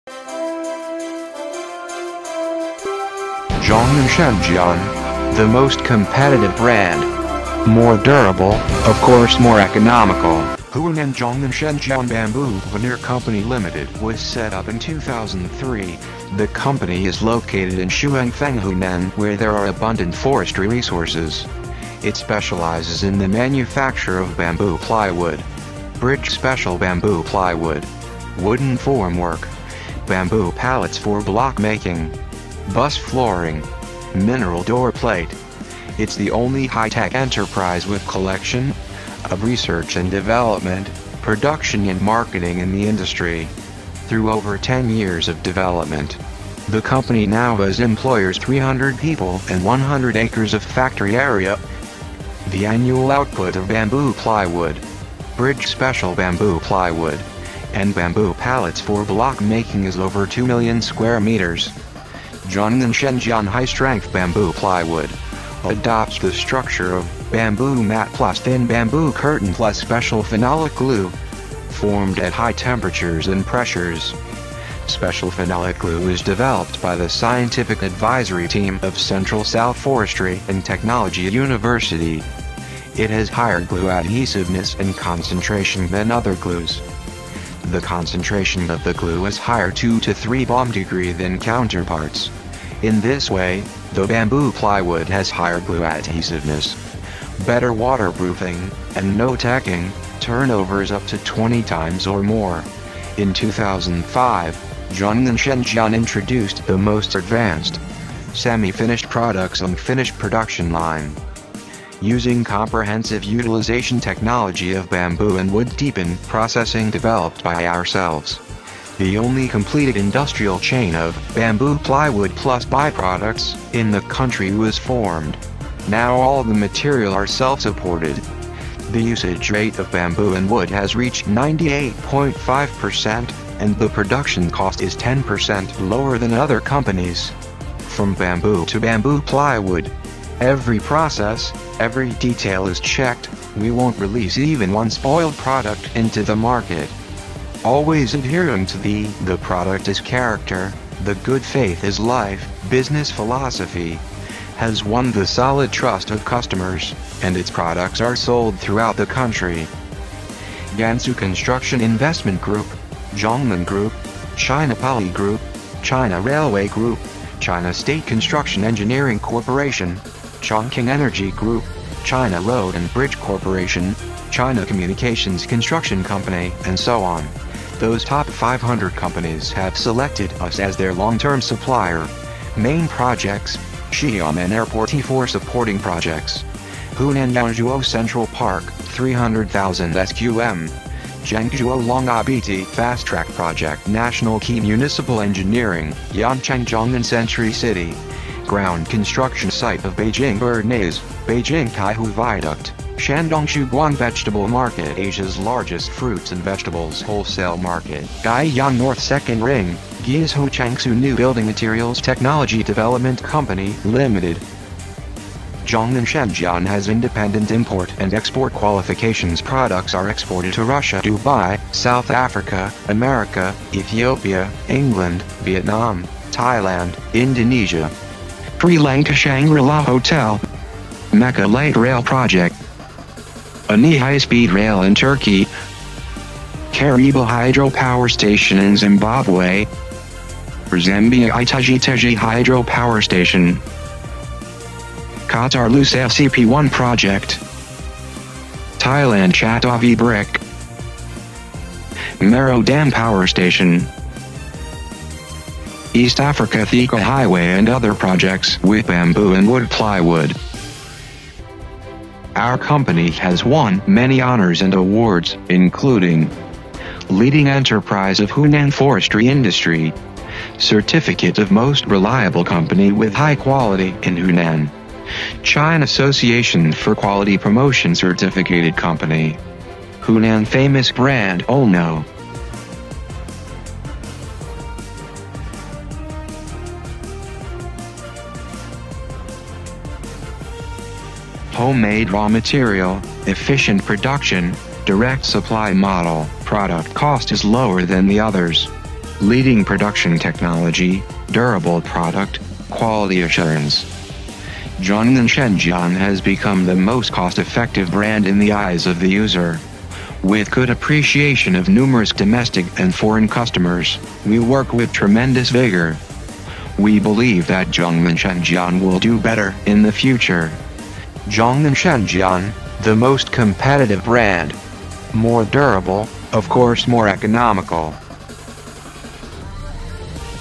how to made bamboo plywood ??
ZNSJ bamboo plywood is are widely used for housing building, bridge, viaduct, dam, tunnel, subway, cast-in-situ concrete application, and also for various containers’ flooring, packing boxes, vehicle and train bottom flooring, stage board and so on. It’s a new generation material of construction ,